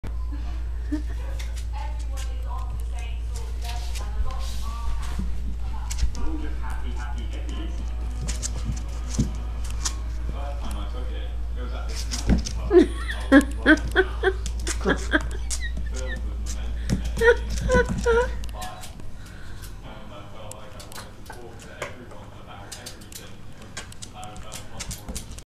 Maggie Plays With Tomato (Moved to new channel)
Maggie is a 8 week old scottie dog puppy playing with a tomato.